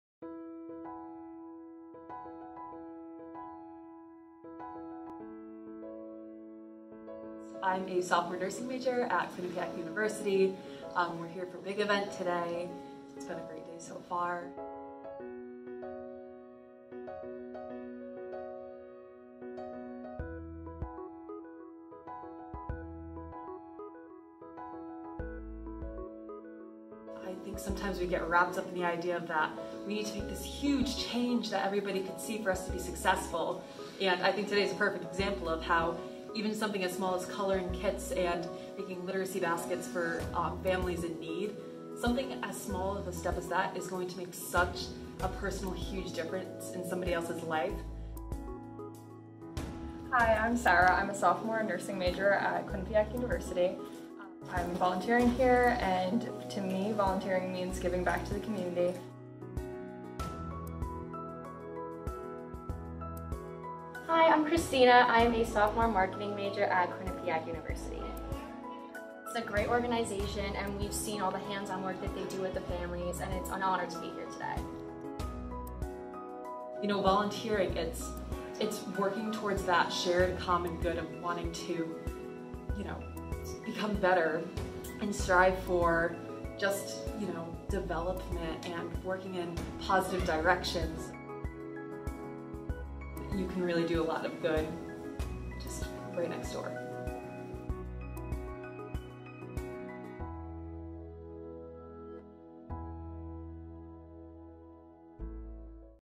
United Way's Quinnipiac Big Event 2021
Volunteers from @quinnipiacu compiled over 50 Literacy Kits for United Way's Early Head Start children and families.

Photography: Maza Rey Photography
Videography: United Way of Greater New Haven